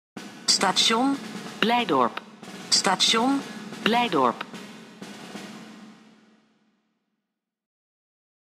Afroep - Metrolijn E - Blijdorp (oud)
De oude afroep op station Blijdorp op metrolijn E.
Deze afroep was te horen tot december 2012, nu niet meer.
Station Blijdorp. Station Blijdorp.